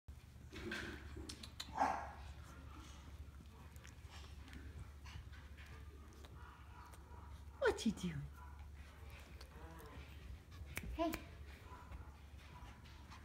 Video of adoptable pet named Malone Papillon
Malone Papillon is an adoptable pet with Recycled Pomeranians and Schipperkes in Garland TX.